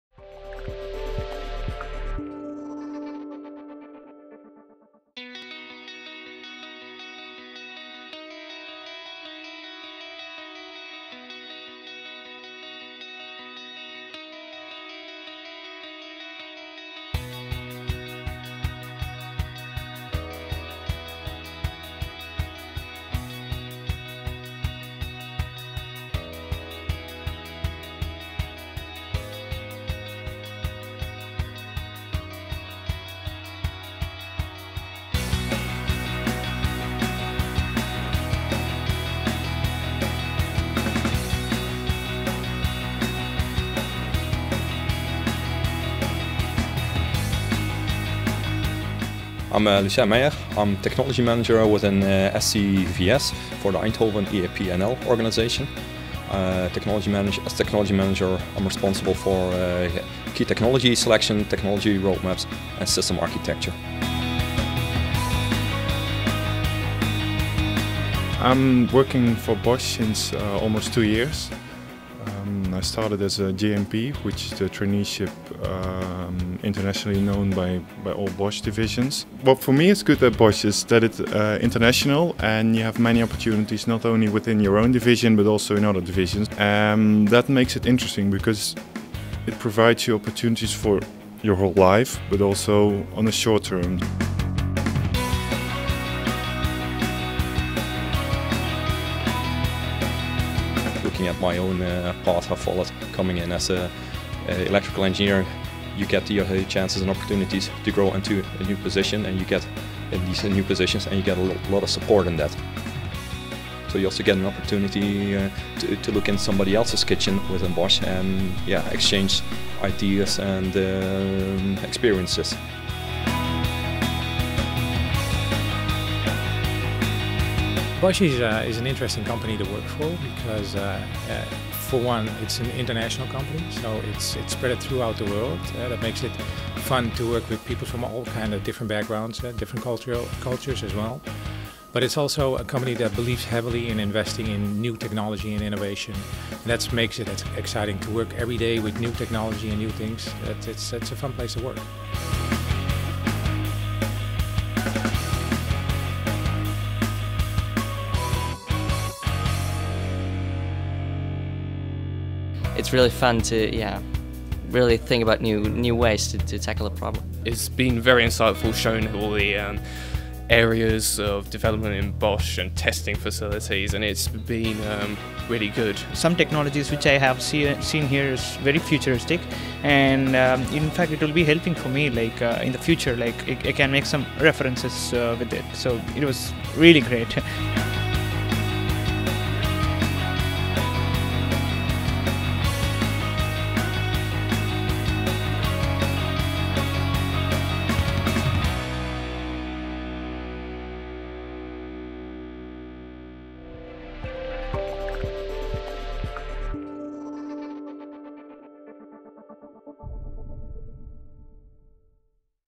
Werken bij Bosch Security Systems in Eindhoven
Inhousedag Bosch Security Systems in Eindhoven voor topstudenten studerend aan een technische universiteit. Het gaat om studenten elektrotechniek, computer science, informatica, werktuigbouwkunde. De dag is georganiseerd door Natural Talent.